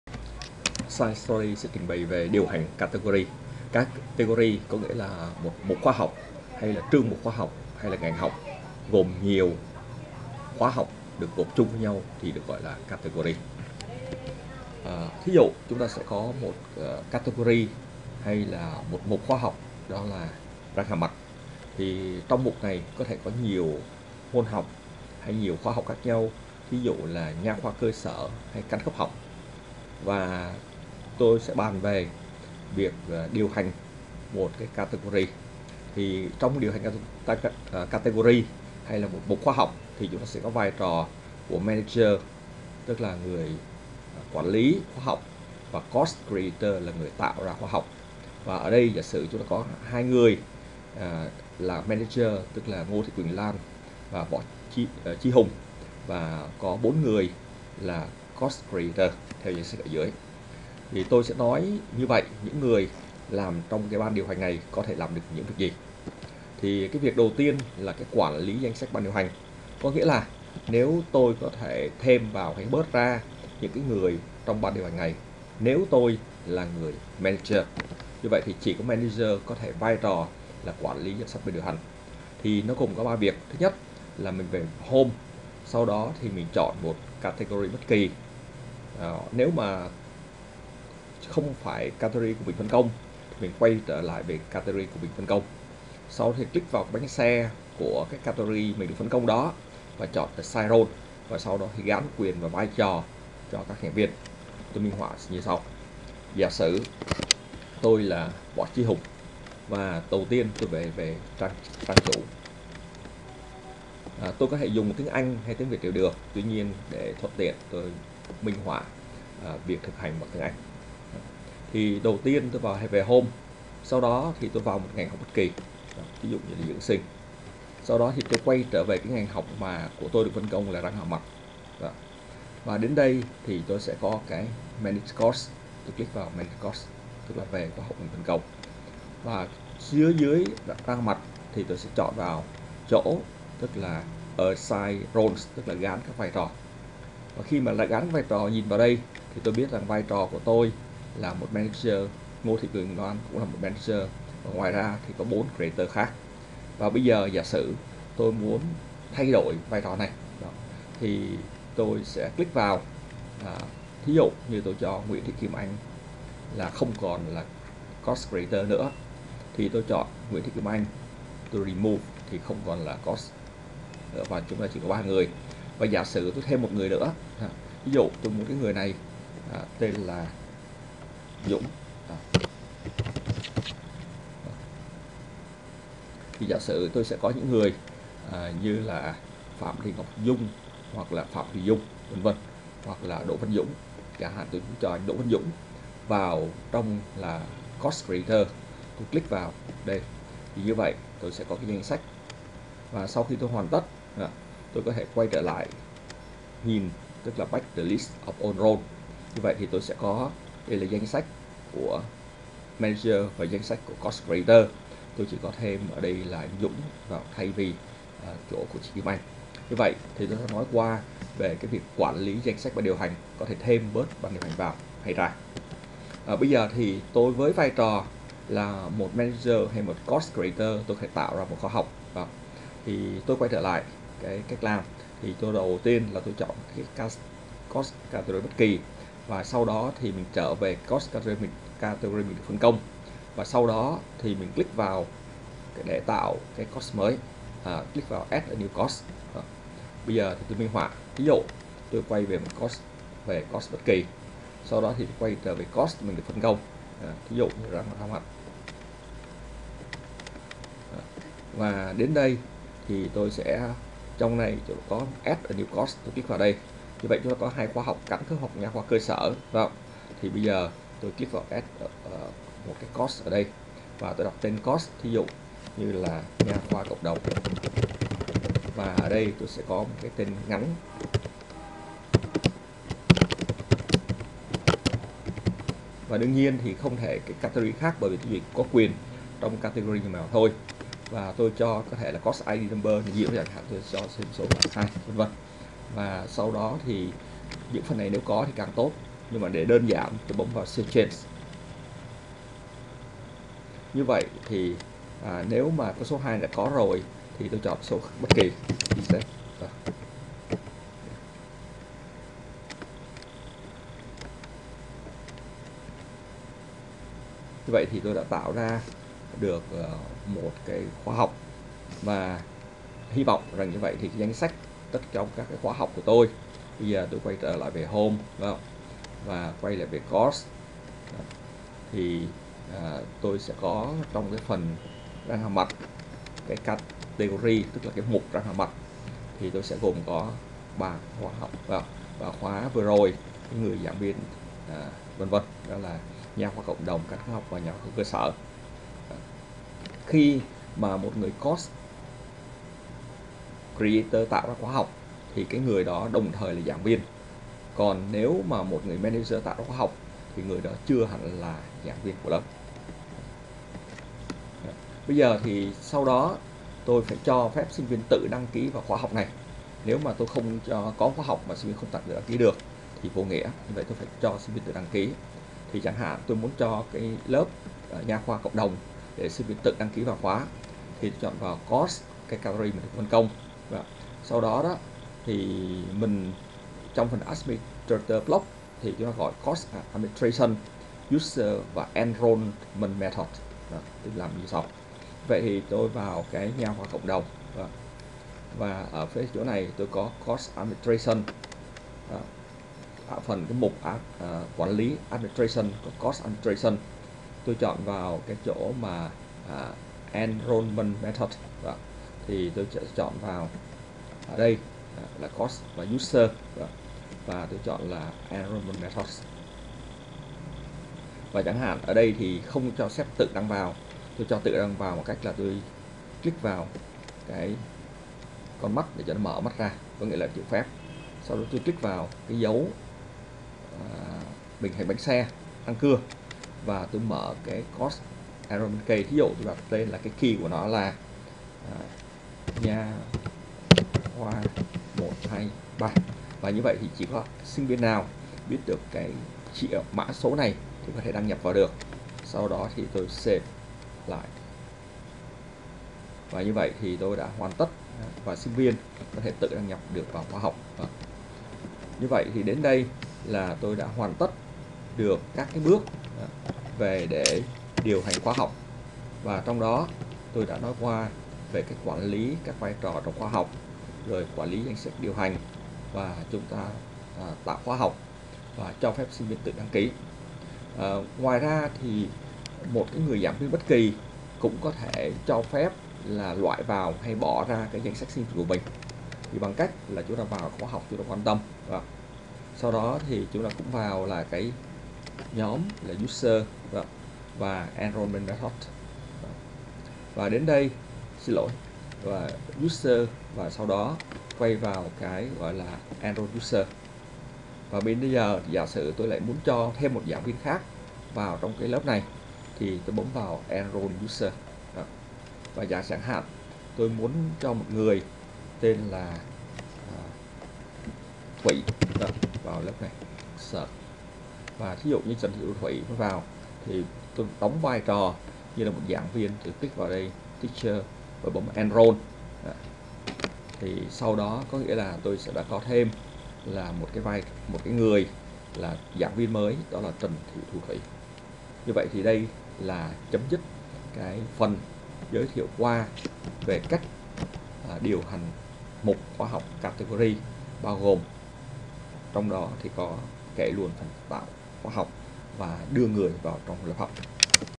Moodle Dieu hanh muc category khoa hoc
Video này trình bày các công việc của ban điều hành một danh mục khóa học (category). Ban điều hành bao gồm Manager và Course Creator. Ban điều hành có thể làm những công việc như quản lí danh sách ban điều hành (*), tạo ra một khóa học mới, cho phép sinh viên tự đăng nhập vào khóa học, quản lí danh sách giảng viên (*) và quản lí danh sách sinh viên của khóa học. Dấu (*) có nghĩa là công việc chỉ do Manager thực hiện và Course creator không thể thực hiện